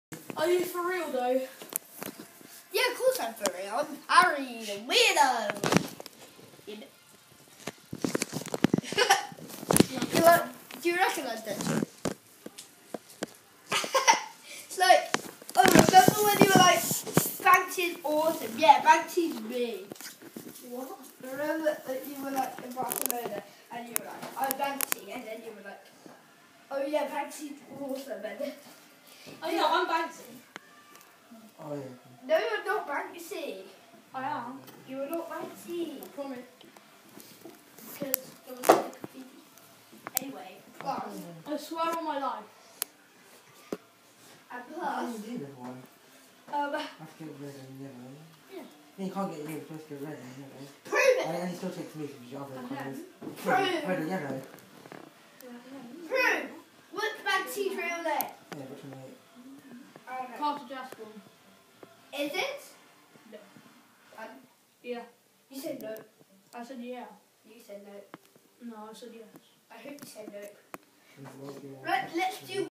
Crafty Cousins when you're not there!
Ever wondered what the crafty cousins are like off camera? Are they still weird? Maybe this will answer your question!